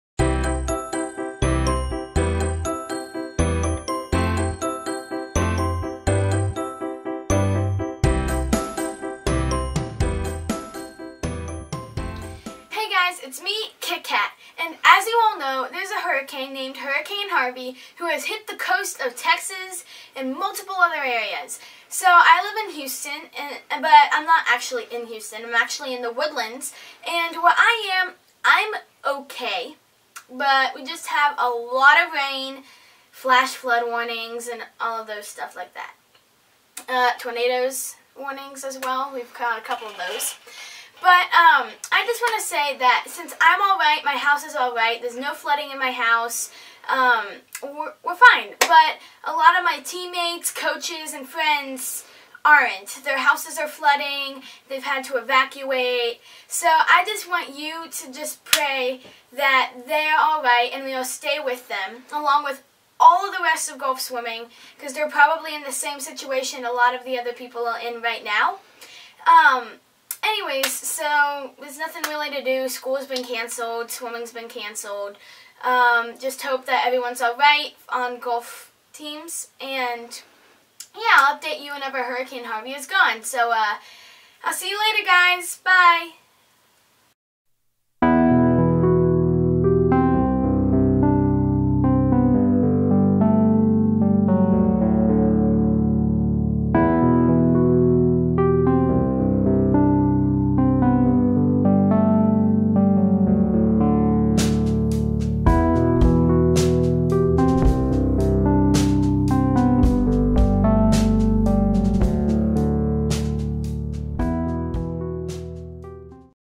Checking in from Texas | Pray for Texas
Hi, checking in from Texas.  Please keep us in your prayers.  I am ok, but I know lots of the Gulf Swimming Family is not.  Keep Praying!